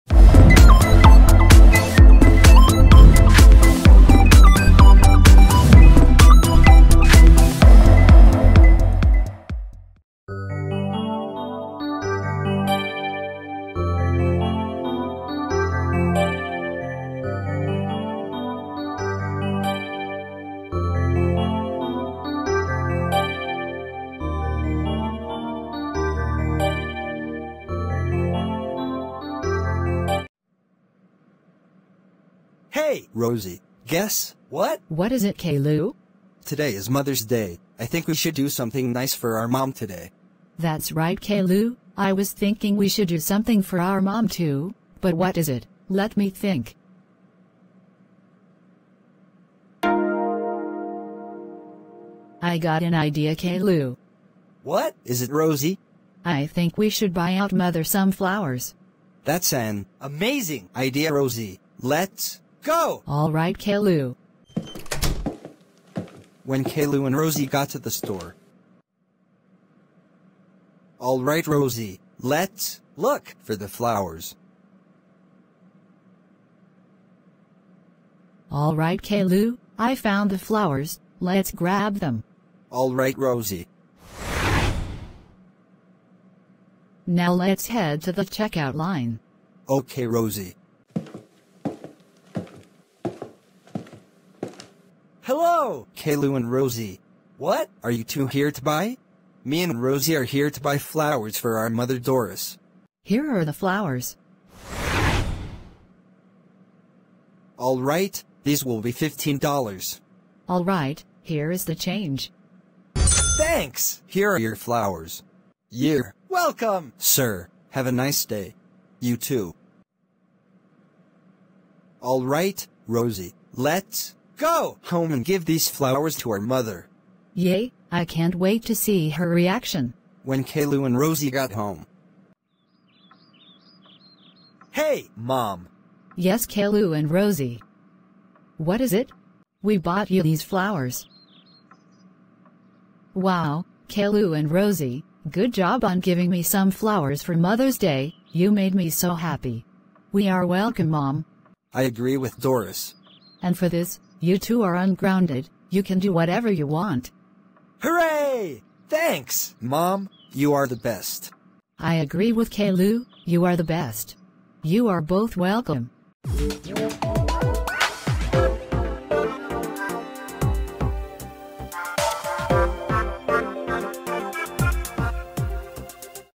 Cailou and Rosie Buy Flowers For Their Mom/Ungrounded (Mother’s Day Special 💐)
Reuploaded due to an error

Happy Mother’s Day To All Mothers Out There 💐